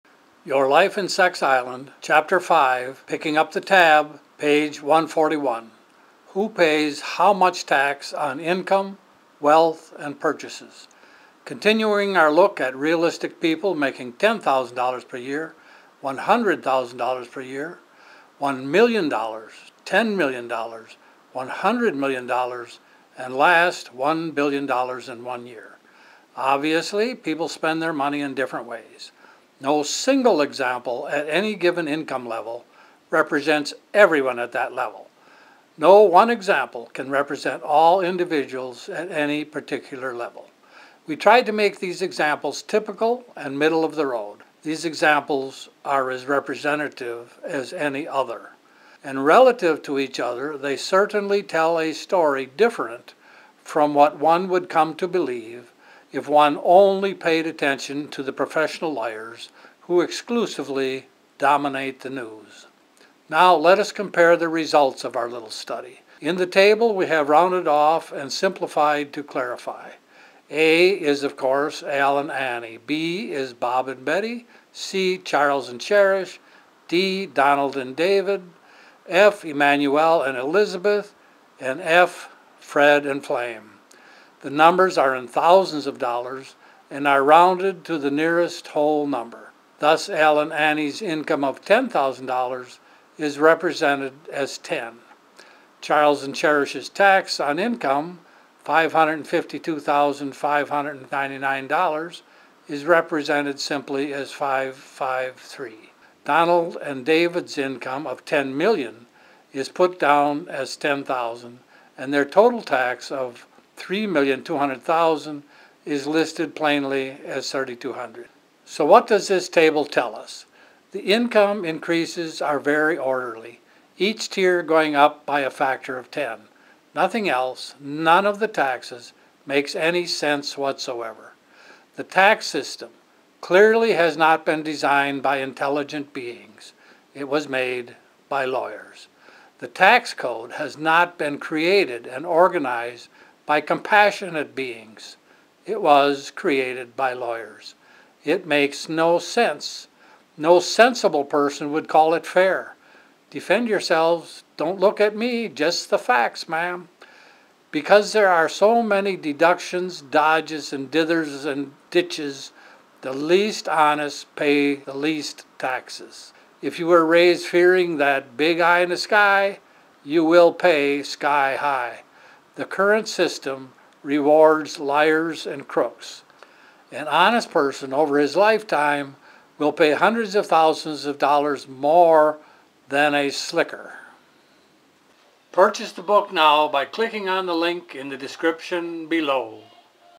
Your Life and Sex Island Chp5 Picking Up the Tab Pg141pa
Who pays how much tax? The tax system rewards liars. Purchase a copy of Your Life and Sex Island here: